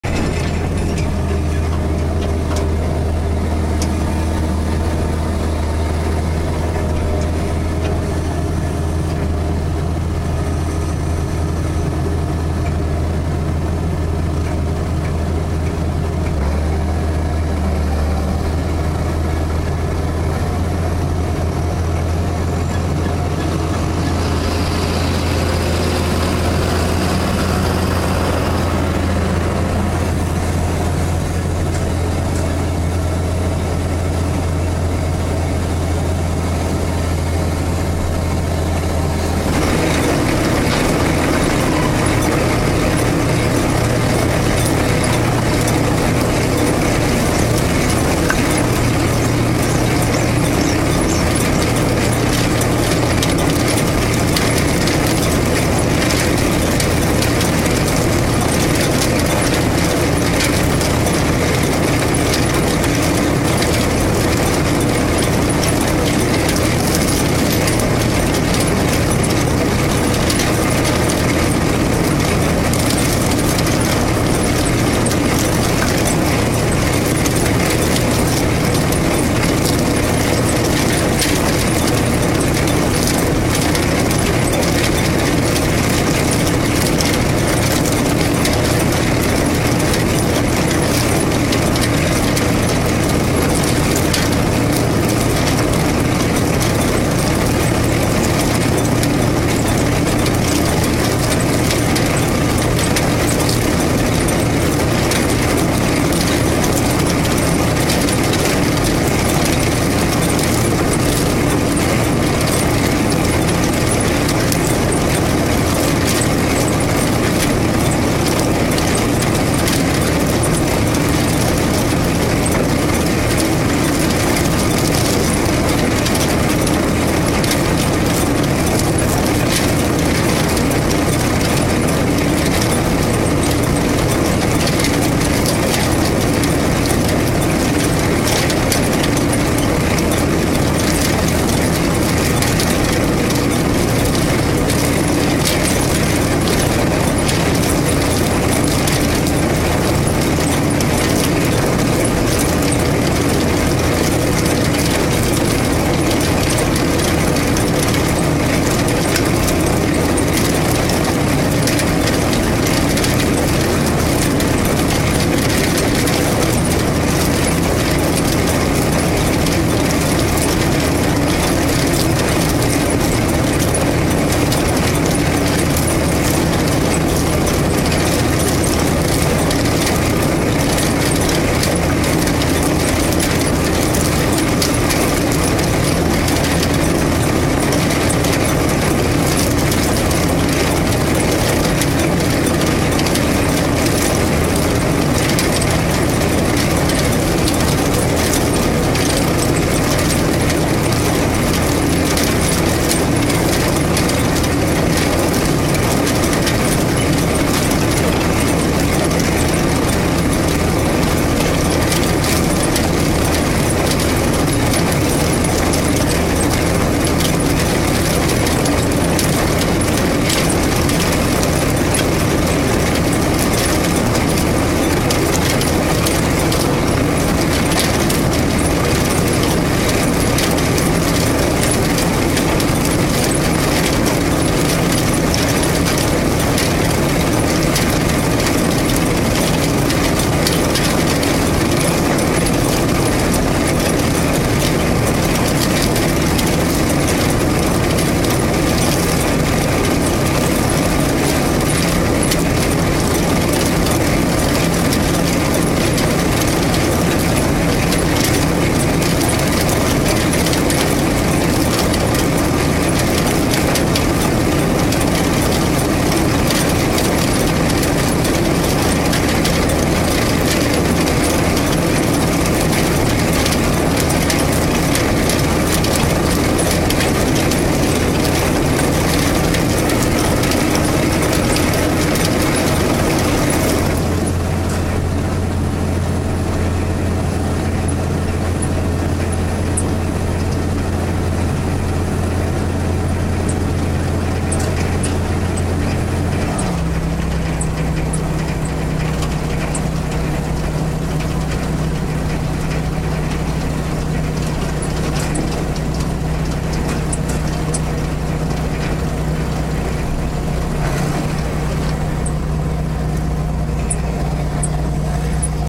360 fly video of potato harvesting
360 video of potato harvest in Thorpdale Australia. With an Underhaug 2600, Cat Challenger 55 and John Deere 6430.
Filmed with the 360 fly.